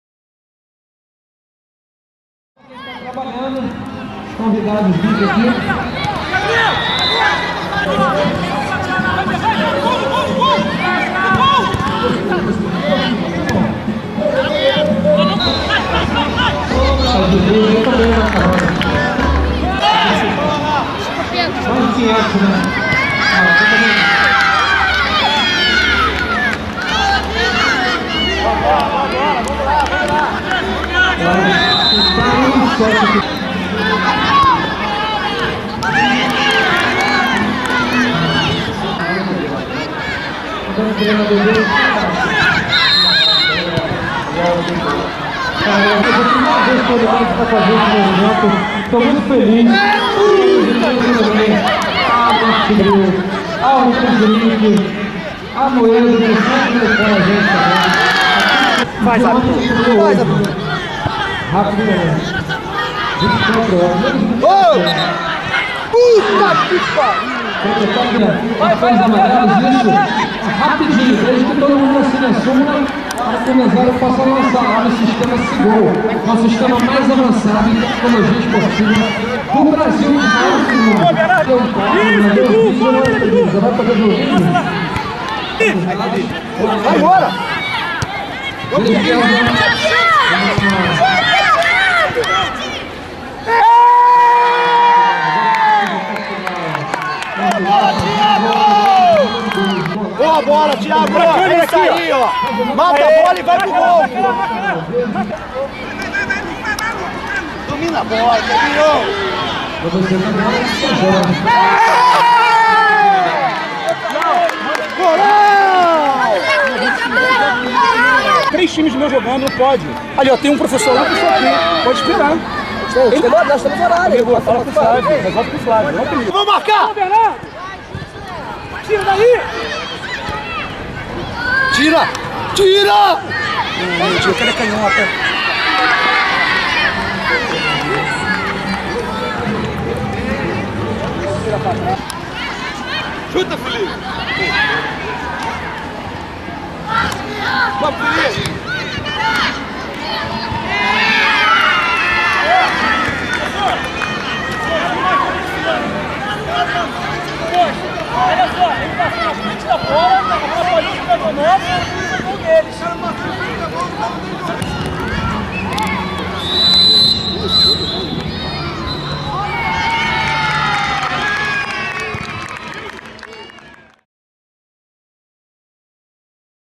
(19/06/10) COSTA DO MARFIM x PORTUGAL (FRALDINHA)
Jogo válido pela Mini Copa do Mundo Unimed.